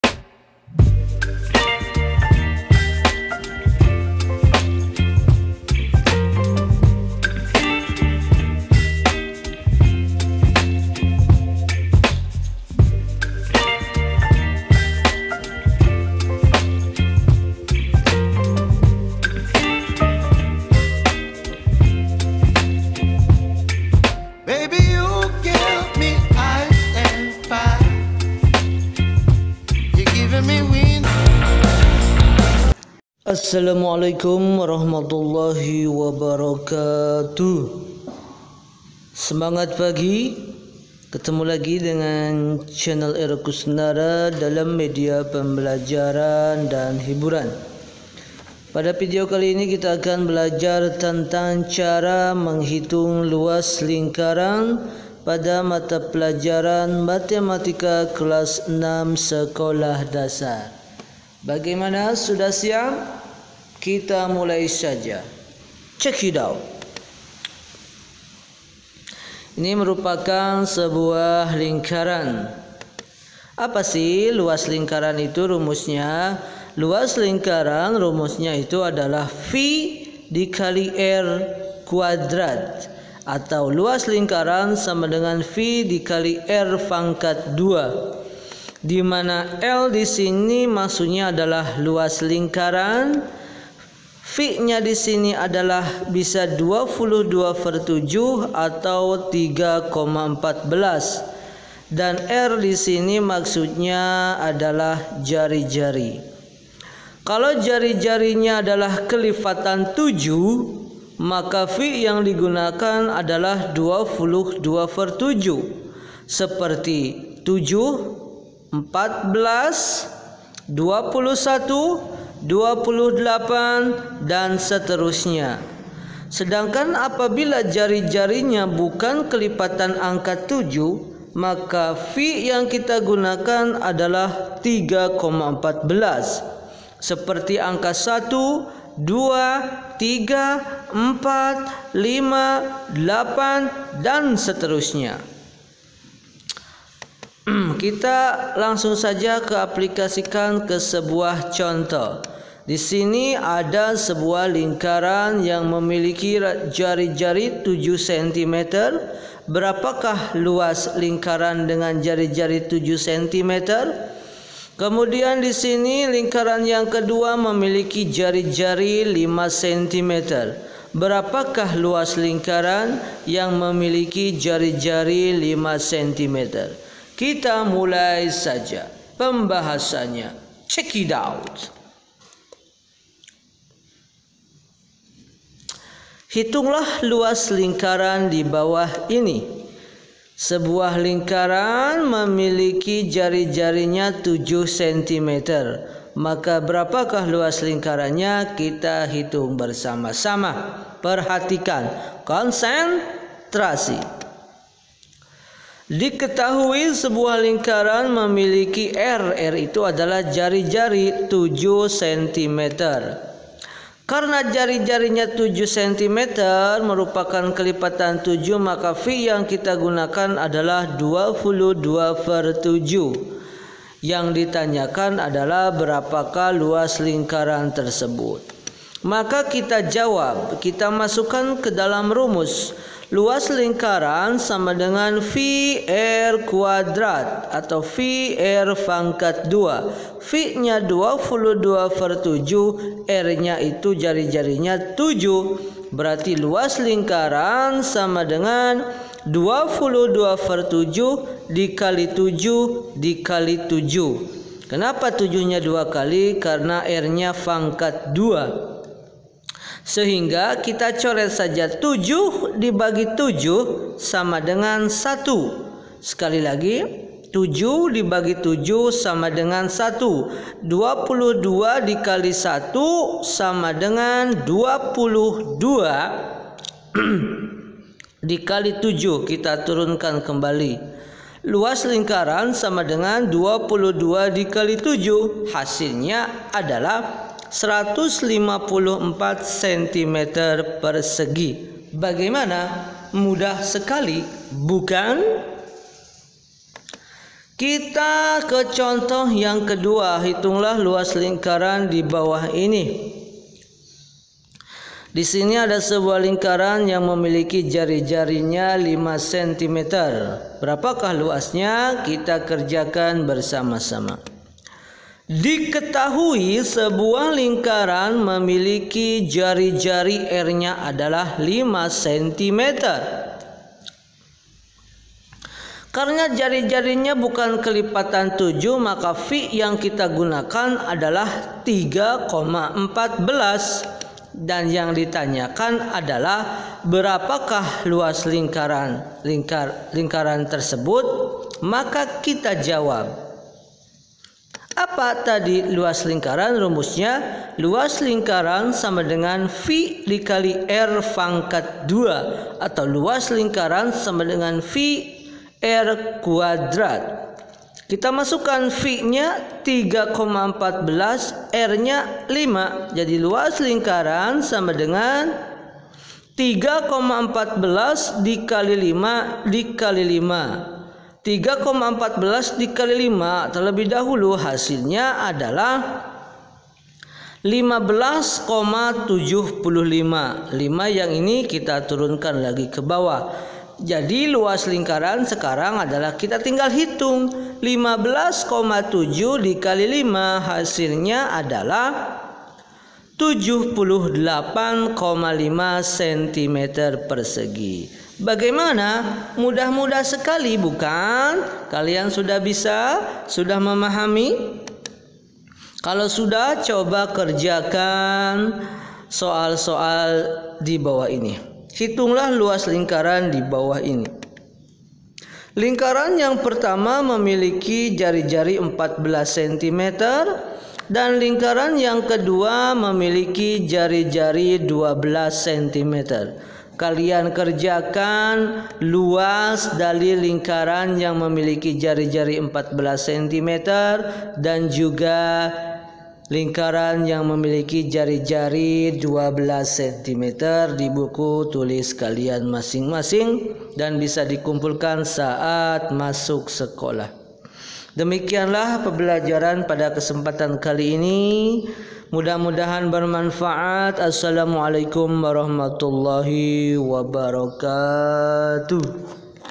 Menghitung Luas Lingkaran Kelas 6 SD
Vidio ini berisi media pembelajaran Mata Pelajaran Matematika yang edukatif, kreatif dan inovatif.
Dalam vidio ini dibahas cara menghitung luas lingkaran yang dipelajari oleh siswa sekolah dasar kelas 6.
Rumus Luas Lingkaran = πr²
Mudah maudahan vidio pembekajaran ini bisa memberikan manfaat untuk kita semua, semoga bermanfaat.